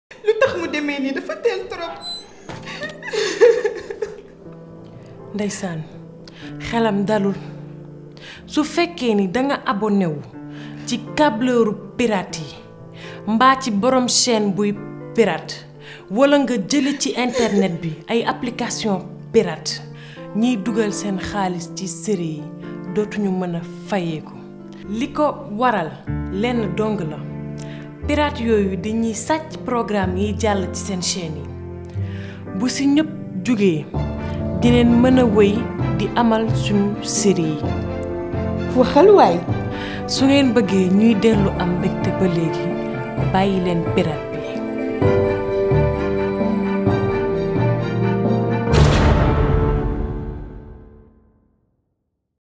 PIRATER SÀCCLA ! (KHALIMA)
Voice Africa signe la campagne 360 anti-piratage de l'association Convergence.Ce film met en scène la stars du petit écran Sénégalais Khalima Gadji. Elle sensibilise toute la famille sur le caractère illégal de la télé via un réseau pirate, quelle que soit la technologie utilisée. Le piratage est dangereux : mauvaise qualité des images, risque d'exposition des enfants à des images violentes ou pornographiques, atteinte néfaste au développement de la culture sénégalaise.

Conception : Voice Africa 2020
Réalisation : Aymeric Austry
Cadrage : Tom Escarmelle
Montage : Ilam Camara
Production : Cécile Duintjer